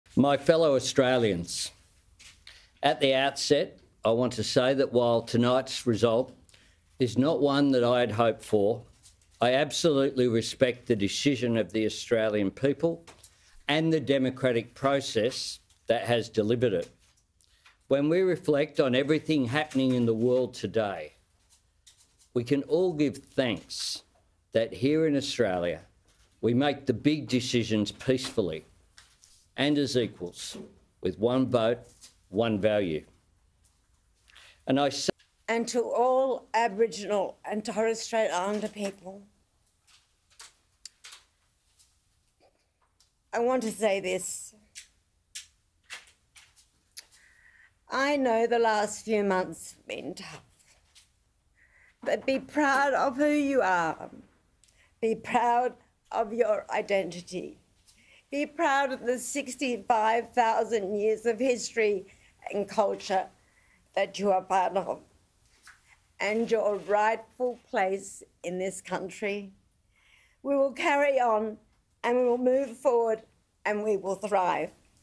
PM and Linda Burney's emotional speeches after the Voice vote fails
PM and Linda Burney's emotional speeches after the Voice vote fails with a 'no' result.